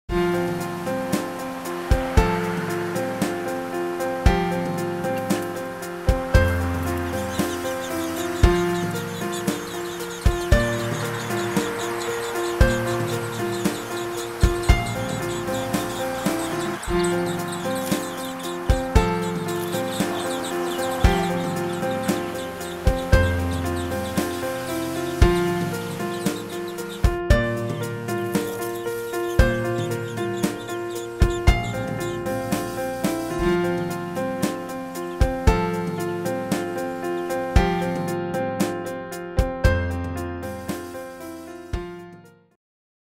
The Robins of North Andover: Worms
Worms, worms and more worms is the theme as this family of robins grows up in North Andover, Massachusetts